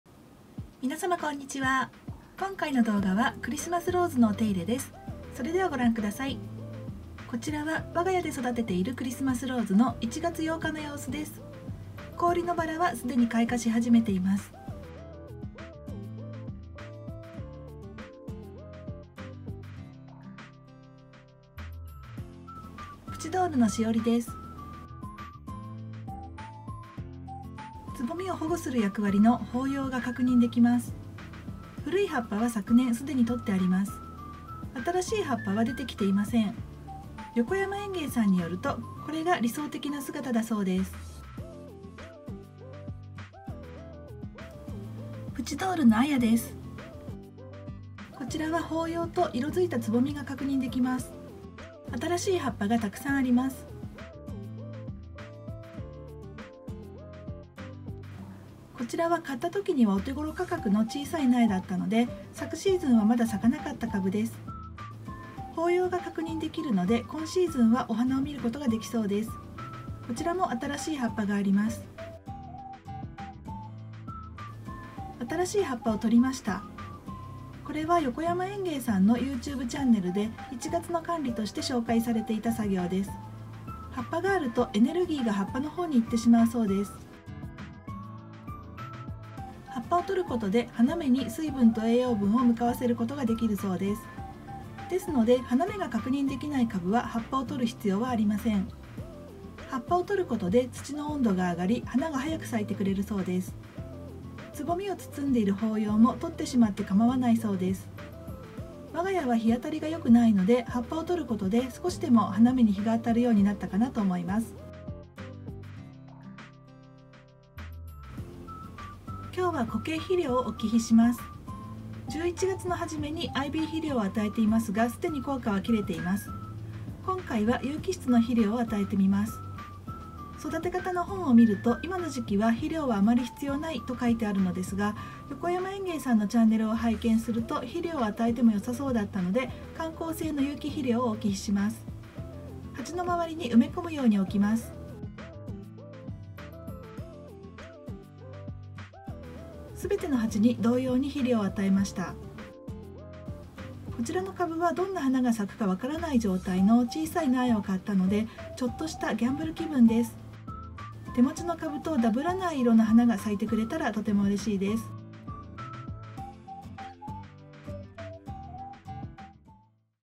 1月上旬クリスマスローズ〇新しい葉っぱを取るメリット〇氷の薔薇開花はじめ〇プチドール〇施肥
古いゴワゴワの葉っぱを取るだけでなく、株によっては、新しい柔らかい葉っぱも取ってＯＫだそうです。そのメリット等。